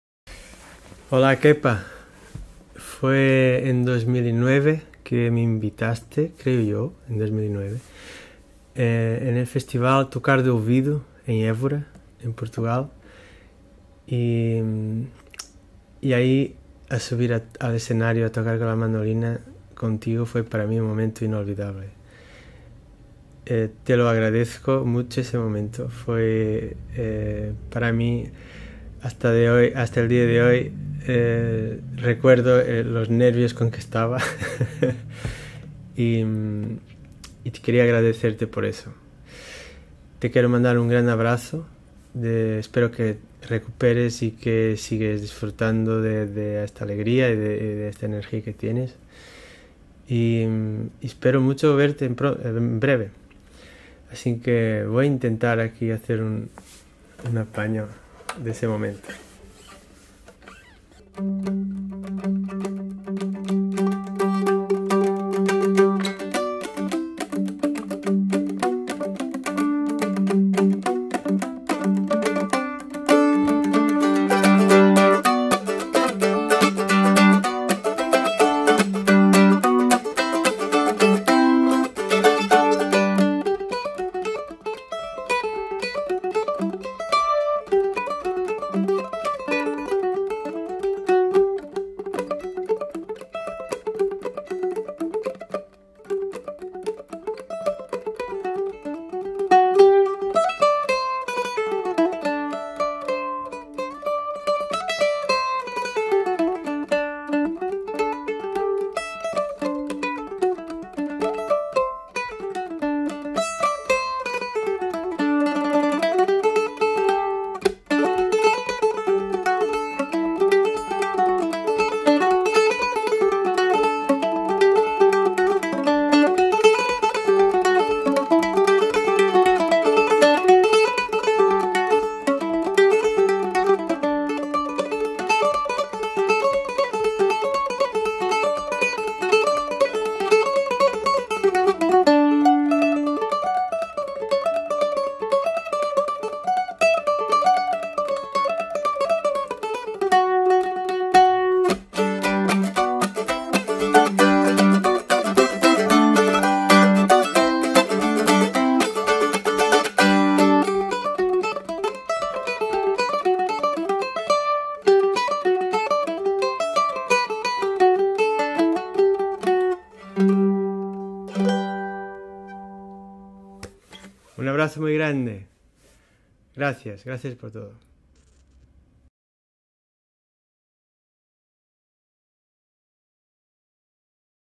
Luis Peixoto
Mensaje número 19 dirigido al admirado artista Kepa Junkera y a todos los amantes de la música.
¡Hoy lo envía Luis Peixoto!